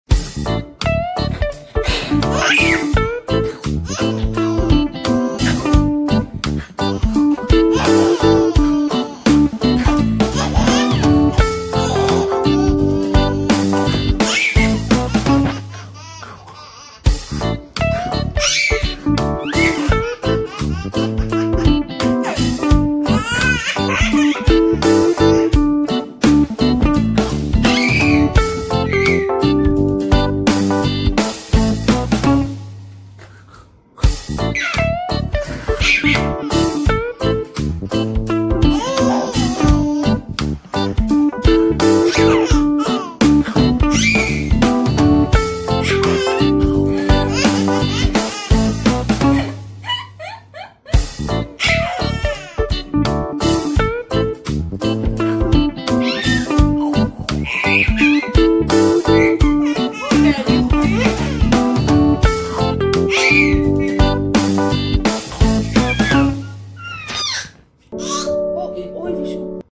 Holdu laughing like hell, dadu as monkey 😁😁😁|cute baby video
-pif8xVkA86DyIoAVQ

KEEP SUPPORTING MY CHANNEL...THANK YOU SO MUCH 😍😍😍